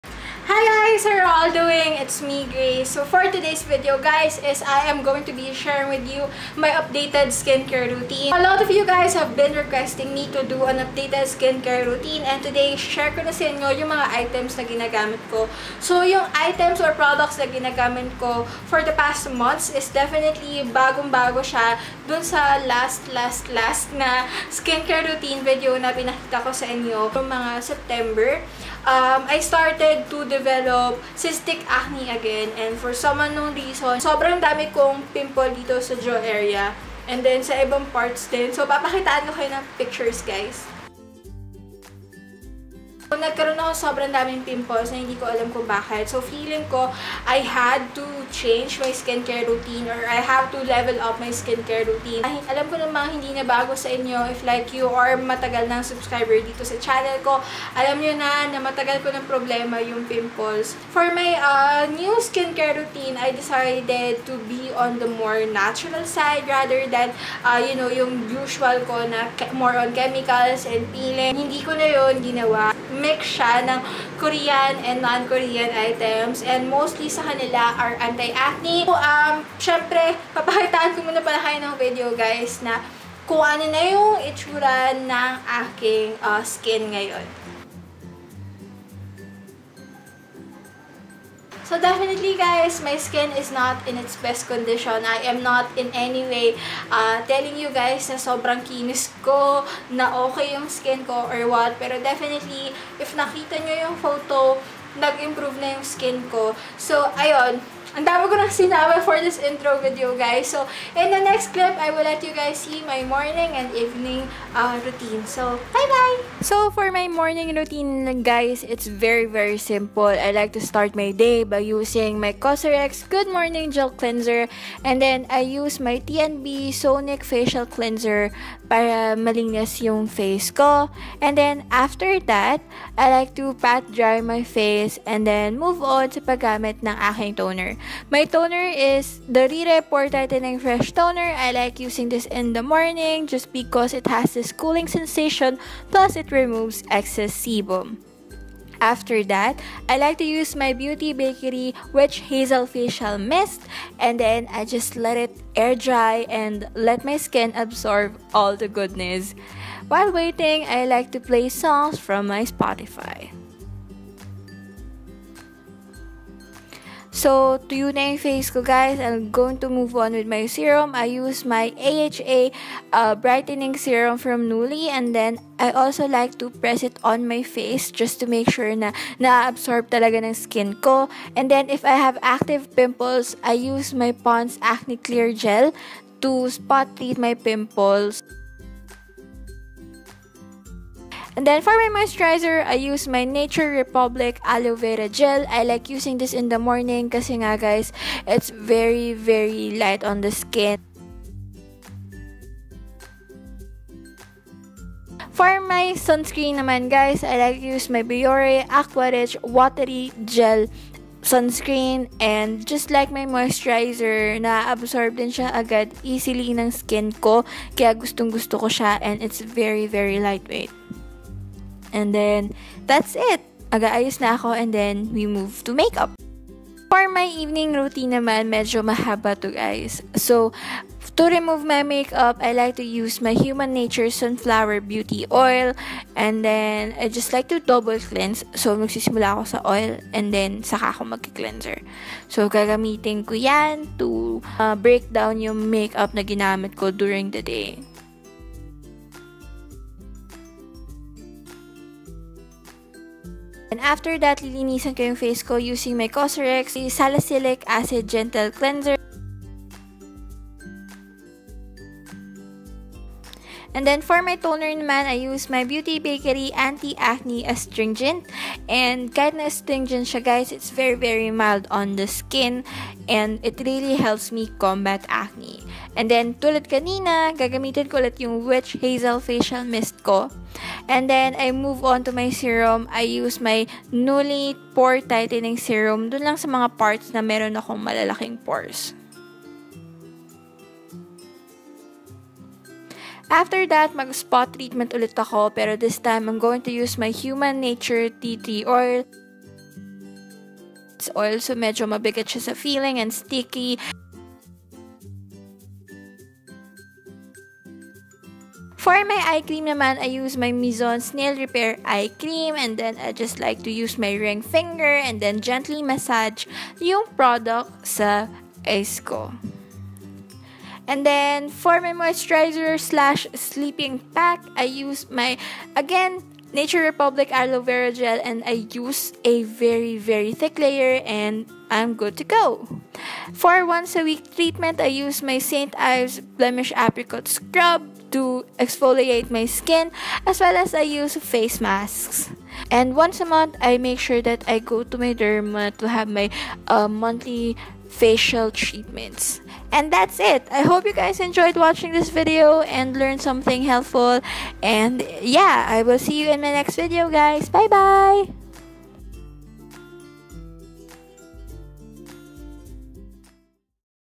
AFFORDABLE Skincare Routine for TIGYAWAT PRONE SKIN (PHILIPPINES)
Hi everyone~

In today's video I share with you my current skincare routine. If you remember, I shared with you last time that I recently had acne breakouts (hormones to blame I guess?) that led me to change my skincare routine. It was I believe, a good move since I've noticed that the quality of my skin changed. I hope that you find something here that can also help you especially if you have acne prone skin.

PRODUCTS MENTIONED:

Cosrx Good Morning Gel Cleanser
Rire Pore Tightening Toner
The Beauty Bakery Witch Hazel Facial Mist
Neulli AHA Vita Brightening Serum
Ponds Acne Clear Leave On Gel
Nature Republic Aloe Vera Gel
Biore UV Aqua rich Water Gel
Human Nature Sunflower Beauty Oil
Cosrx Salicylic Acid Gentle Cleanser
Nuelli BHA Pore Tightening Serum
Mizon Snail Repair Cream
Human Nature Tea Tree Oil
St. Ives Apricot Scrub
Foodaholic 4D Face Masks

EDITOR: Adobe Premiere CC